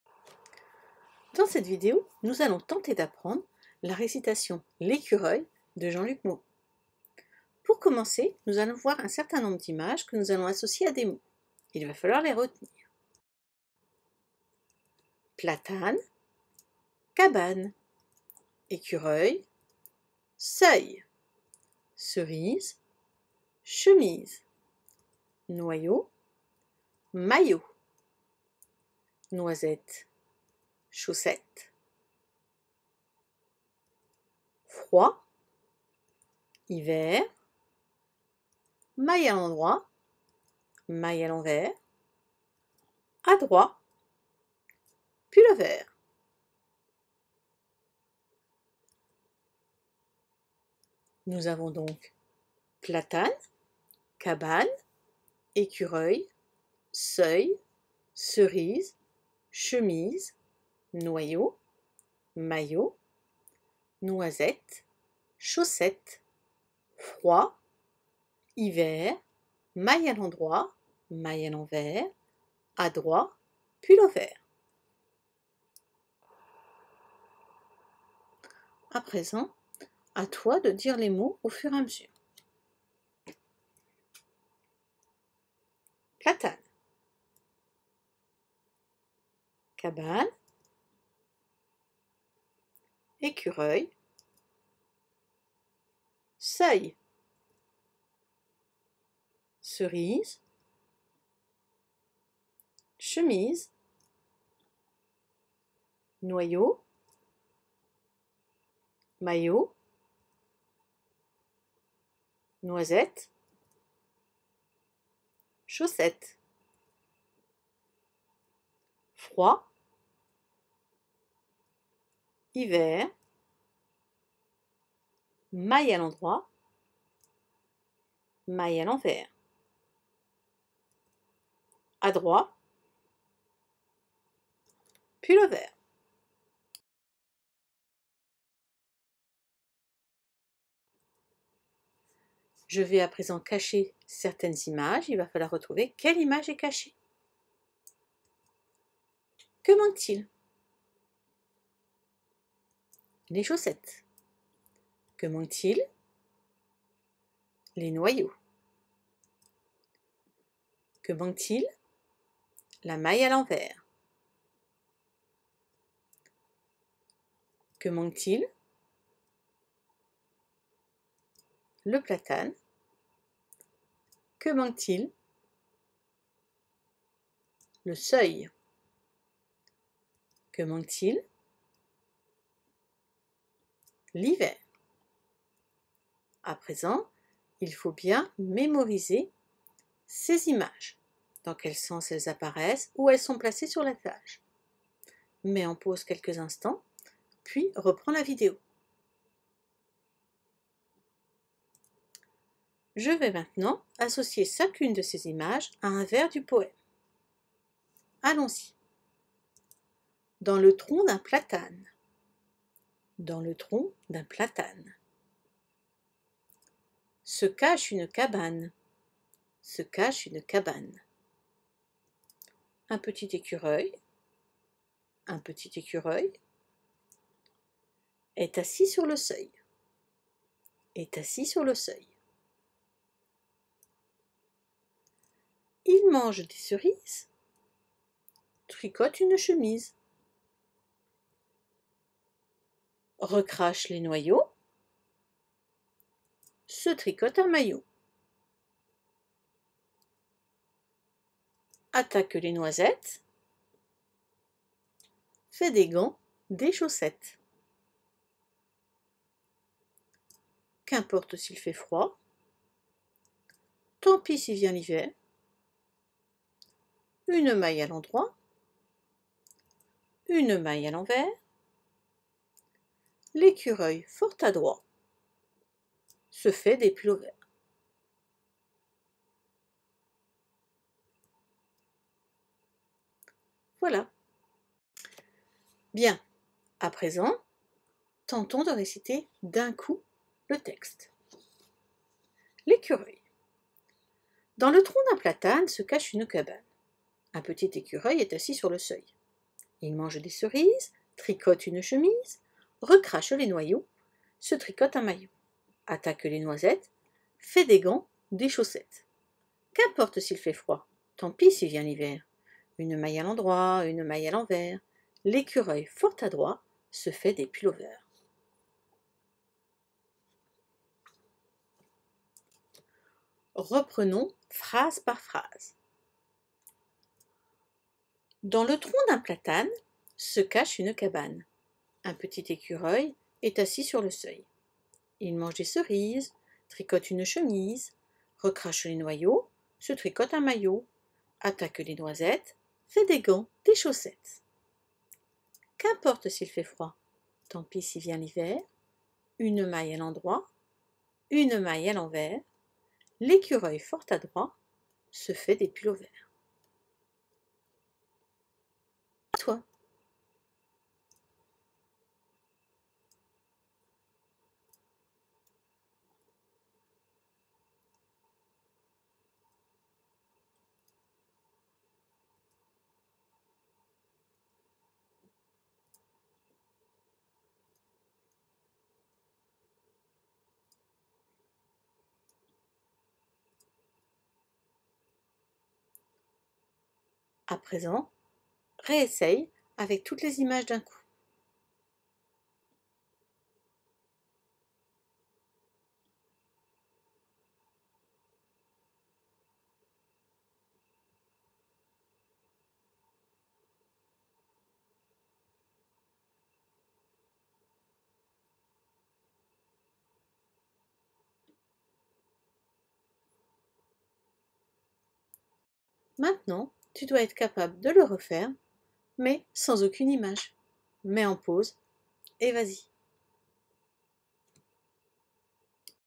Récitation: l'écureuil de Jean-Luc Moreau
Vidéo d'apprentissage: comment retenir une poésie? Comment travailler lorsqu'on a une mémoire visuelle ?